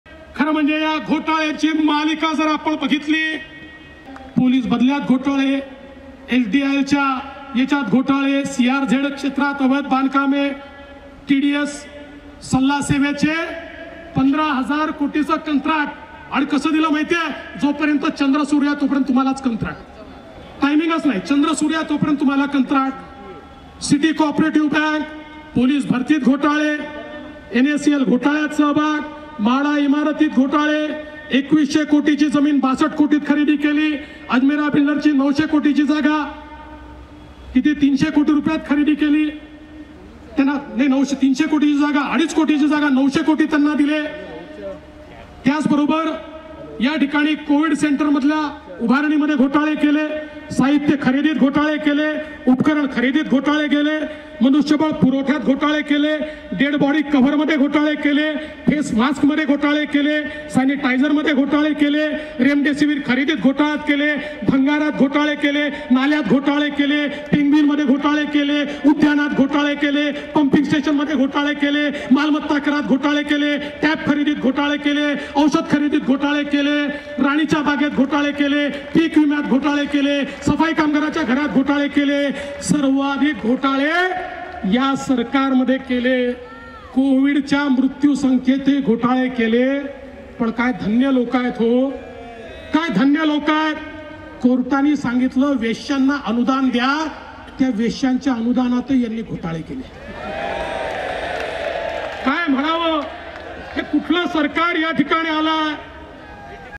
महाविकास आघाडीचा वेश्यांच्या अनुदानातही घोटाळा... । मुंबई । देवेंद्र फडणवीस
(महाराष्ट्र सन्मान सोहळा सभा । मुंबई । दि. 1 मे 2022)